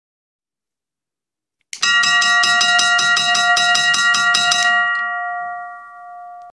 Mijn eigen mechanische overwegbel//My own mechanical RR-crossing bell
Sinds deze week ben ik de gelukkige bezitter van een echte elektromechanische bel! Vandaag even getest en korte video gemaakt.

Since this weak, I'm the happy owner of a real mechanical railroad crossing bell! Today I've tested the bell and it works perfect!